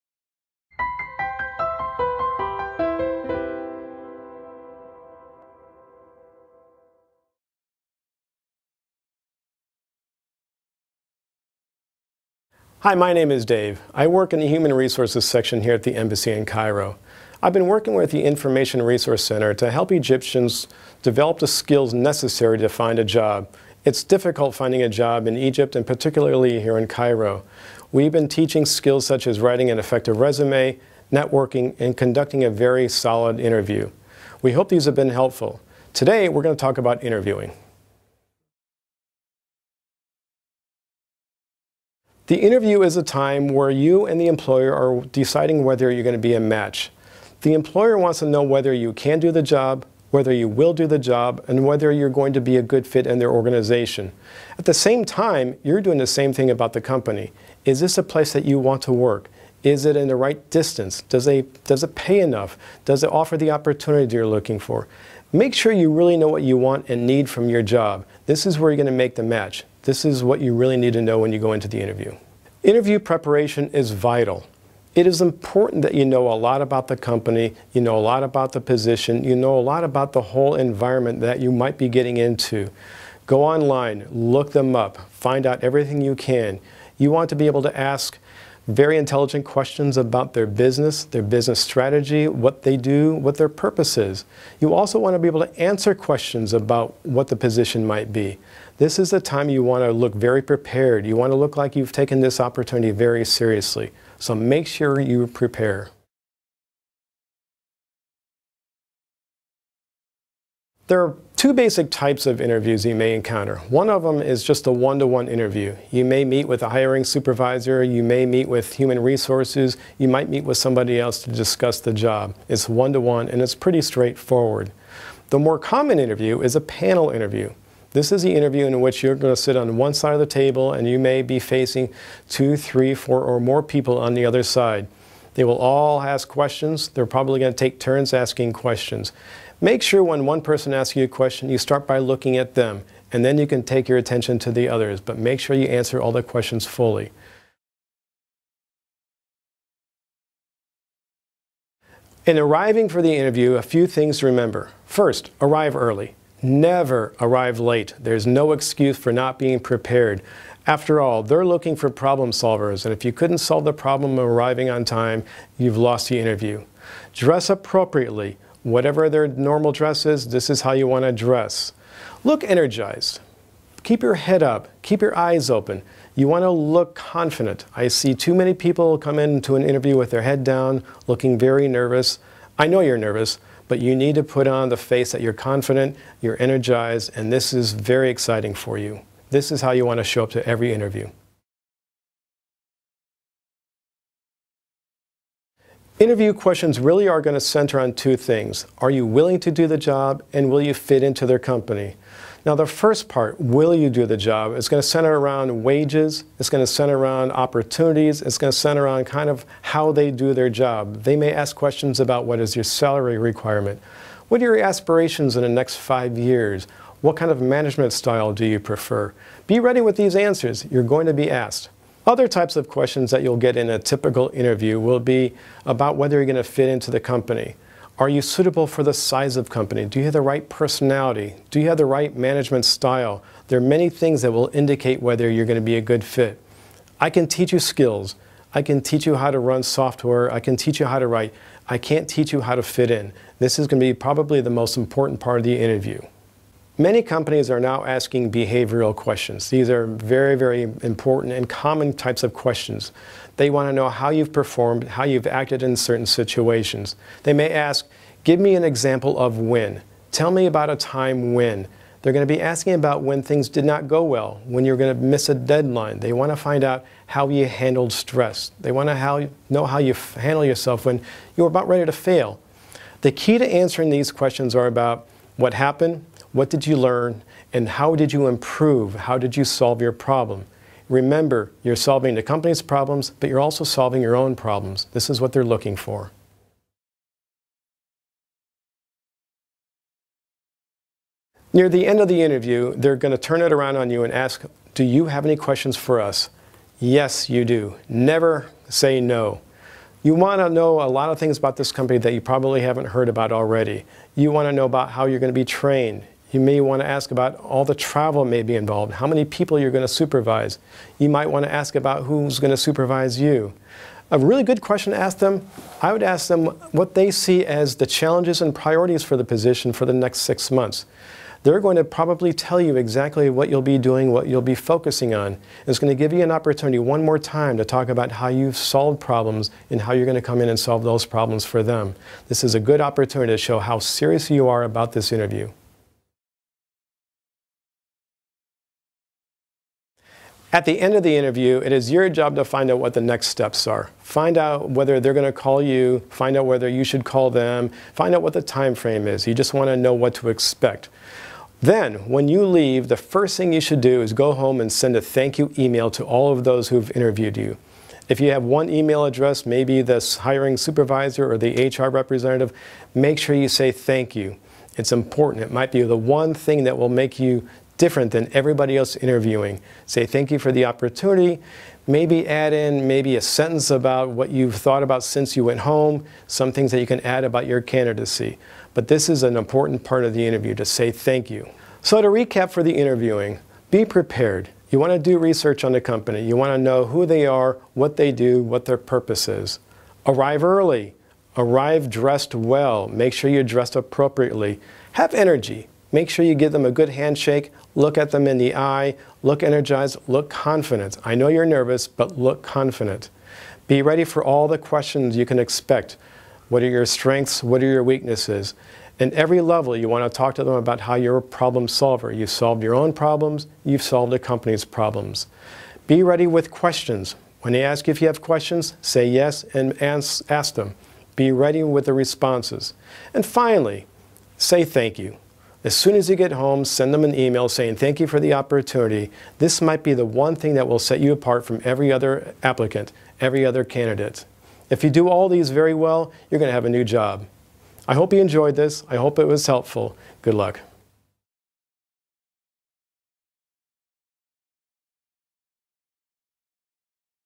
David Reiter - Job Interview
Do you want to improve your job interviewing skills? Want to make a good impression in a job interview? Learn tips from one of our own Human Resource experts on how to succeed in a job interview. Watch the video for a summary of the topics that we have covered in the session held at the Embassy’s Information Resource Center on January 4, 2016.